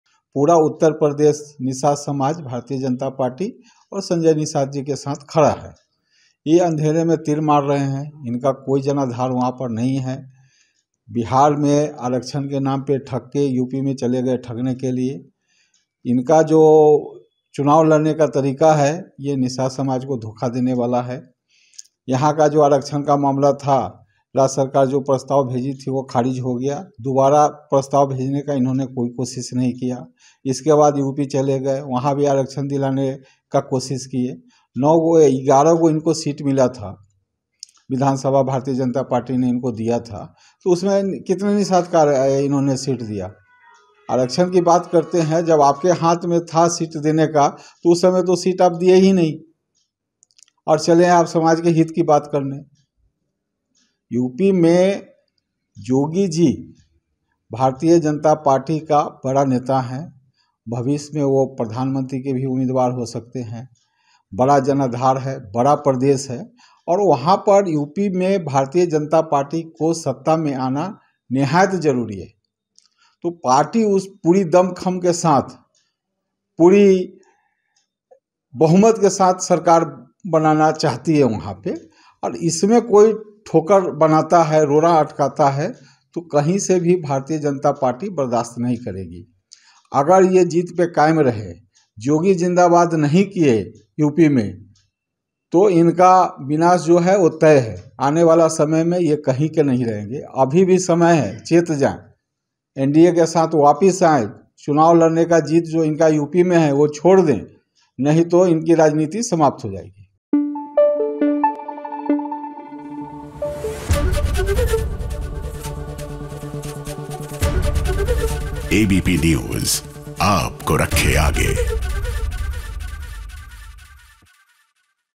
Ajay Nishad की Mukesh Sahani को नसीहत, ‘यूपी में नहीं किया योगी जिंदाबाद, तो विनाश तय’
बीजेपी सांसद अजय निषाद ने वीआईपी अध्यक्ष और बिहार सरकार में मंत्री मुकेश सहनी को नसीहत देते हुए कहा कि योगी भावी प्रधानमंत्री है और अगर यूपी में उन्होंने योगी जिंदाबाद नहीं किया तो राजनीति में उनका विनाश तय है.